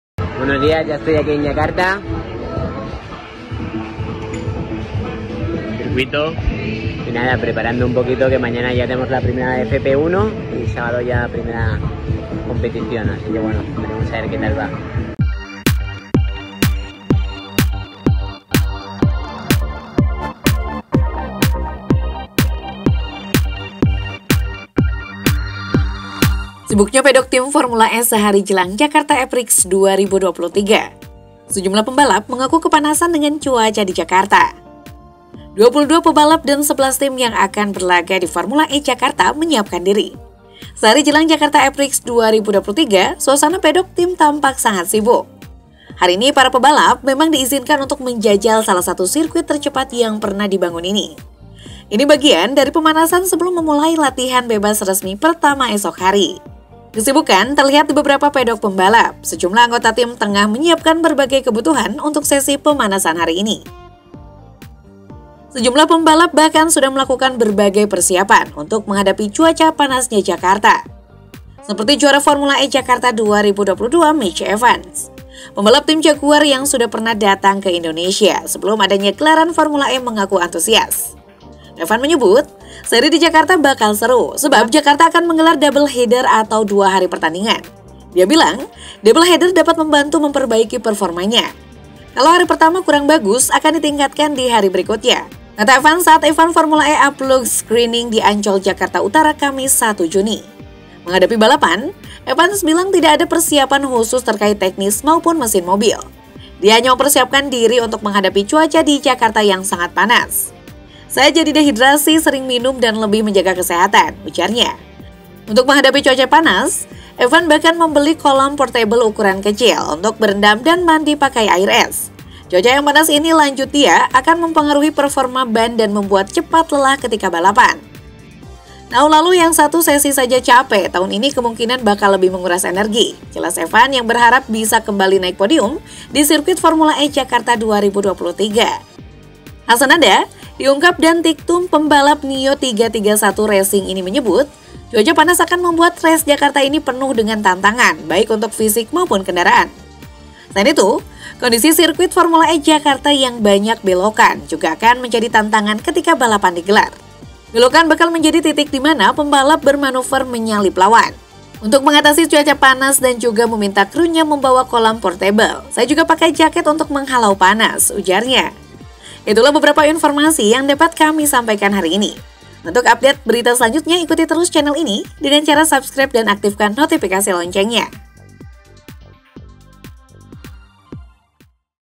PAGI INI ⚡ PADDOCK FORMULA E JAKARTA MULAI SIBUK 😍 PEMBALAP MENGELUH PANASNYA JAKARTA
Sibuknya Paddock Tim Formula E Sehari Jelang Jakarta Eprix 2023.
Senumlah pembalap mengaku kepanasan dengan cuaca di jakarta.

22 pebalap dan 11 tim yang akan berlaga di Formula E Jakarta menyiapkan diri. Sehari jelang Jakarta Eprix 2023, suasana paddock tim tampak sangat sibuk.
Hari ini, para pebalap memang diizinkan untuk menjajal salah satu sirkuit tercepat yang pernah dibangun ini. Ini bagian dari pemanasan sebelum memulai latihan bebas resmi pertama esok hari.
Kesibukan terlihat di beberapa paddock pembalap. Sejumlah anggota tim tengah menyiapkan berbagai kebutuhan untuk sesi pemanasan hari ini.

Sejumlah pembalap bahkan sudah melakukan berbagai persiapan untuk menghadapi cuaca panasnya jakarta. Seperti juara Formula E Jakarta 2022, Mitch Evans. Pembalap tim Jaguar yang sudah pernah datang ke Indonesia sebelum adanya gelaran Formula E mengaku antusias. Evans menyebut, seri di Jakarta bakal seru. Sebab Jakarta akan menggelar double header atau dua hari pertandingan. Dia bilang, double header dapat membantu memperbaiki performanya.
"Kalau hari pertama kurang bagus, akan ditingkatkan di hari berikutnya," kata Evans saat even Formula E "Unplugged Screening" di Ancol, Jakarta Utara, Kamis (1/6).